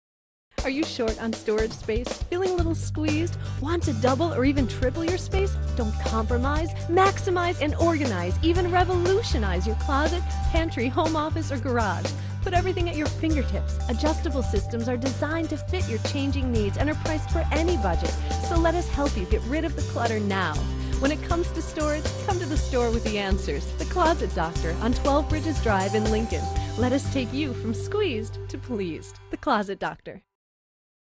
Closet Doctor TV Commercial
30 second tv commercial for closet organizers, garage cabinet and home office systems by The Closet Doctor in the Sacramento CA area.